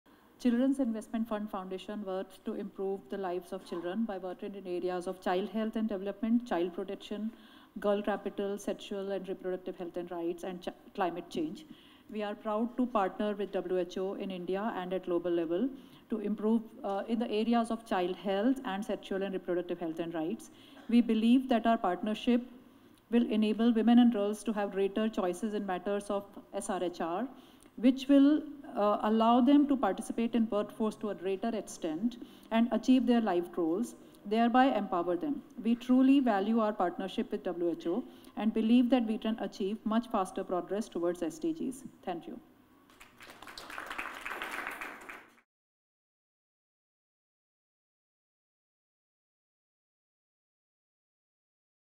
Investment Round (RC77) - Kirti Iyengar, Children's Investment Fund Foundation
Listen in what Kirti Iyengar, Children's Investment Fund Foundation, shared at the investment round of WHO South-East Asia's Seventy-seventh Session of the WHO Regional Committee.